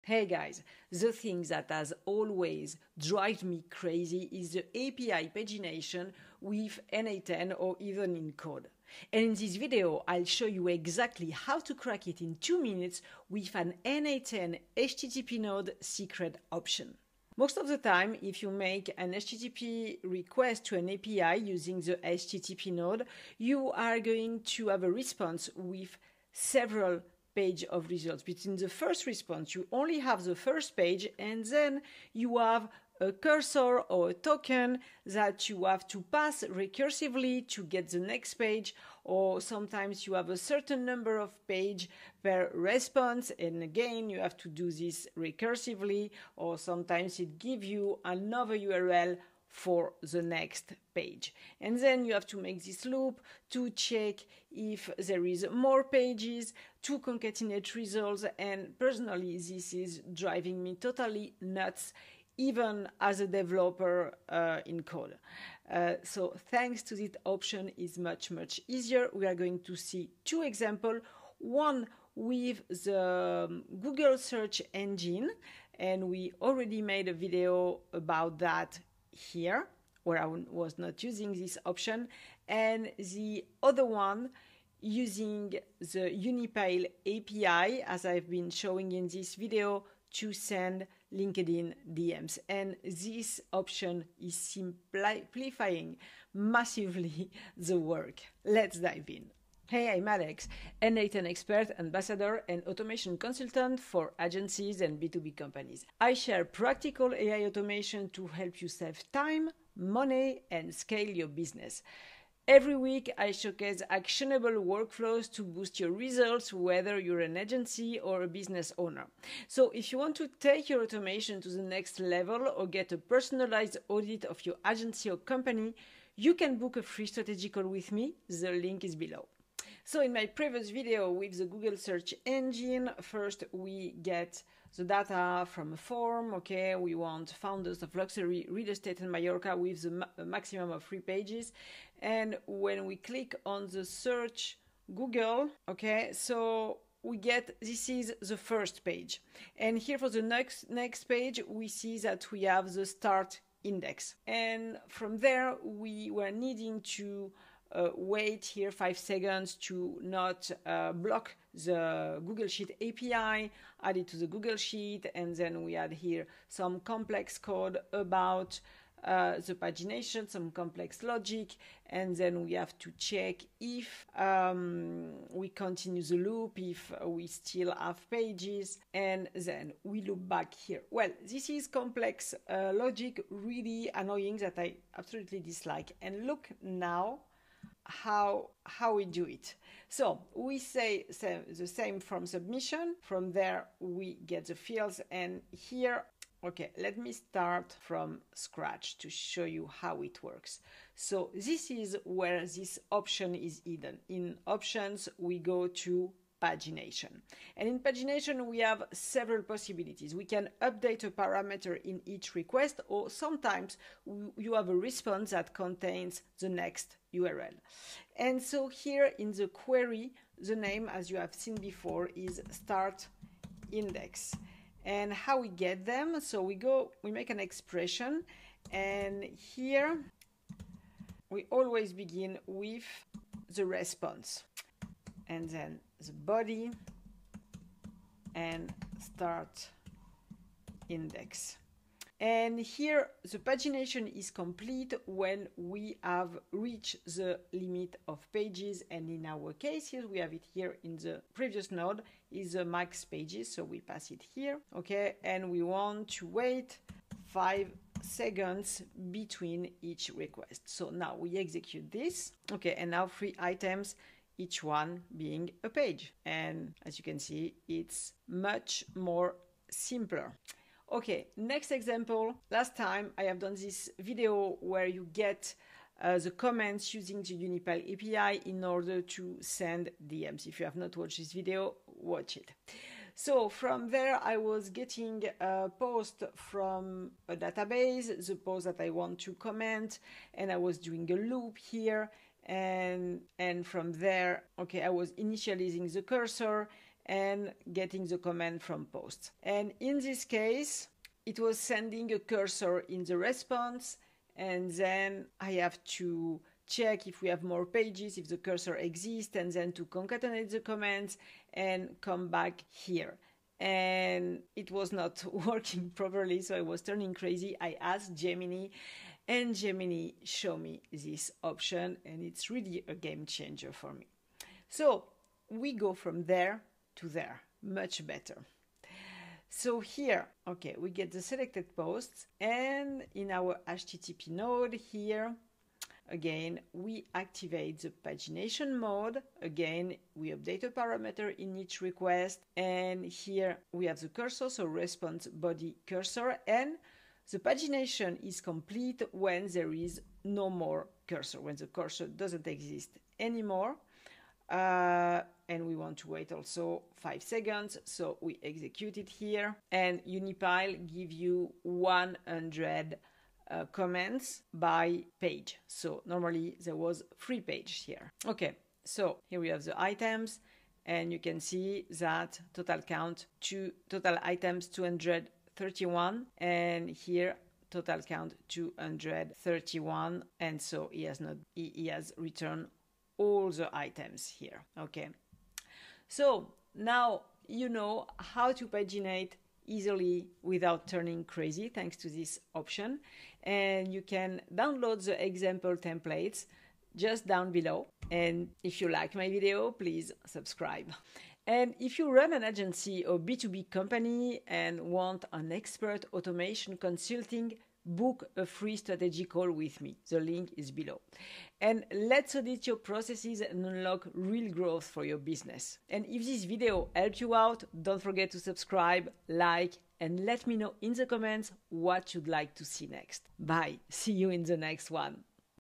n8n API Pagination Solved in Minutes
Pagination doesn’t have to be painful. In this video, I show you step by step how to handle cursors, tokens, and next-page URLs using n8n’s HTTP node pagination feature. With two real examples—Google Search and Unipile—you’ll see how to pull complete datasets without custom scripts. Copy the workflow, plug in your API, and save hours every week.

00:00 – API Pagination Problem in n8n
01:30 – Google Search API Example
03:20 – Easy Pagination Setup in n8n
05:00 – Unipile API Example with Cursor
06:30 – Fetch All Data Automatically
07:30 – Steps